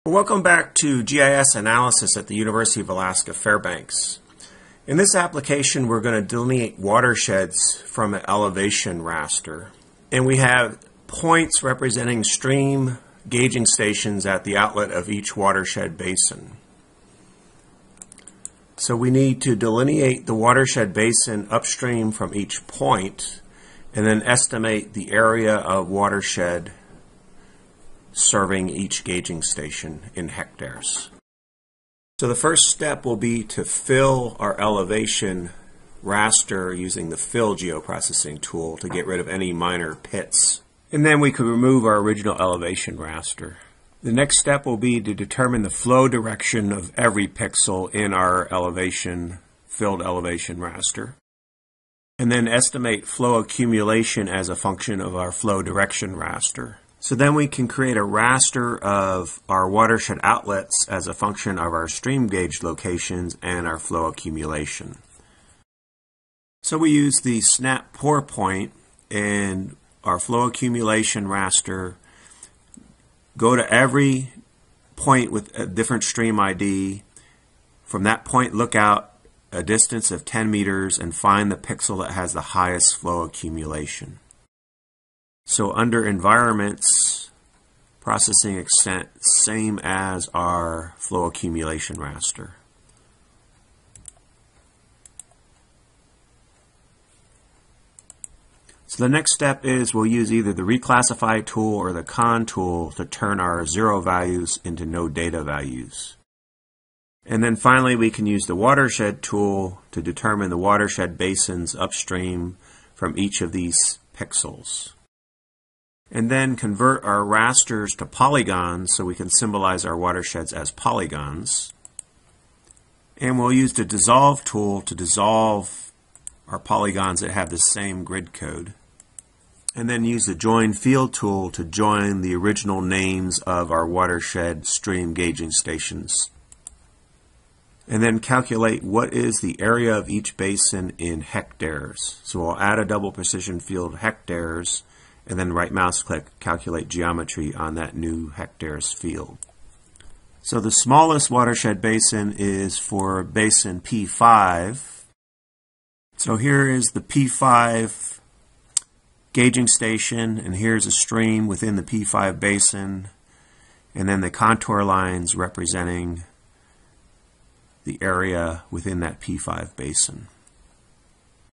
Delineating Watershed Basins Upstream From Gauging Stations
For each of ten stream gauging stations, determine the watershed basin upstream from the stream gauge.